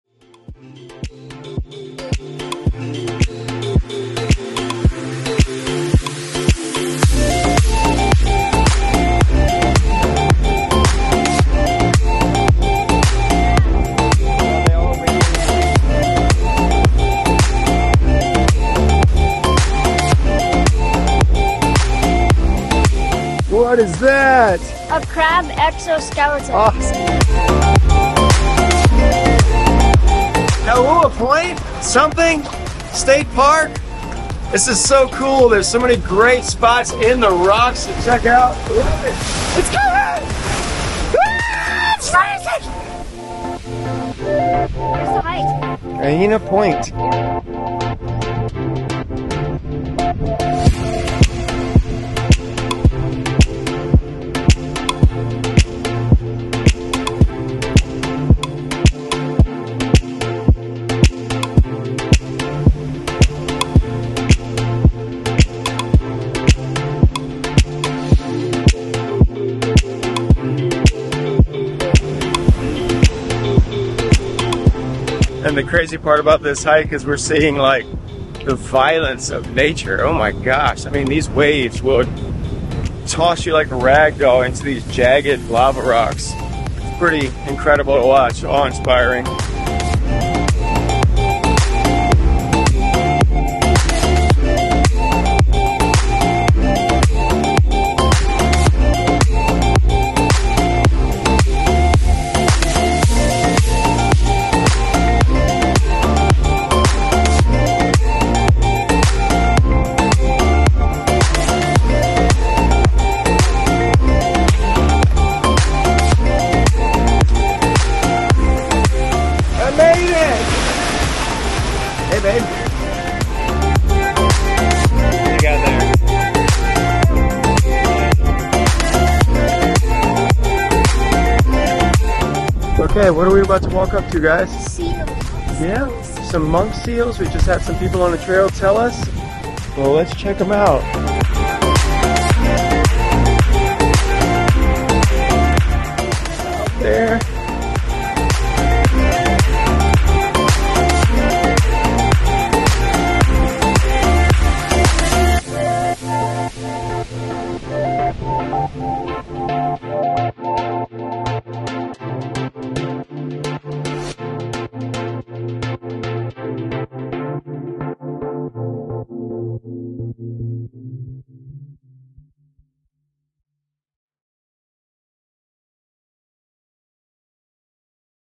Kaʻena Point Trail hike | We almost tripped over Monk seals! | Oʻahu, Hawaii
What an epic family trip to Hawaii!  Here’s a video with some quick highlights from Kaʻena Point Trail. Shot in 4K on iPhone, DJI Osmo Action 3, and DJI Air 2s.